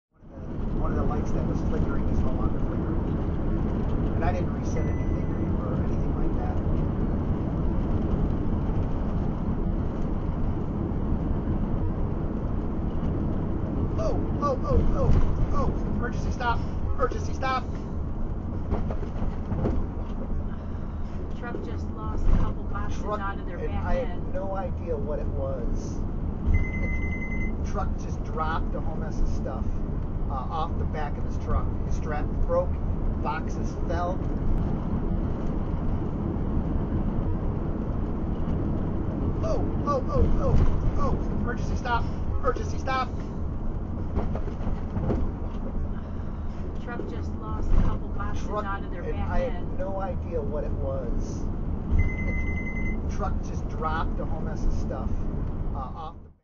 Quick Deceleration of a 45 Foot Tiffin Allegro Bus - No Panic Involved :)
This is a quick deceleration when a truck dropped boxes off the back of it. Went from 65Mph to 20Mph in 3 seconds!  The coach weighs 42,000 lbs and we are towing a Jeep grand Cherokee weighing approximately 5,100 lbs. Having a supplemental braking system in the tow vehicle greatly helps the deceleration in an emergency stop! We use the SMI Air Force Braking System.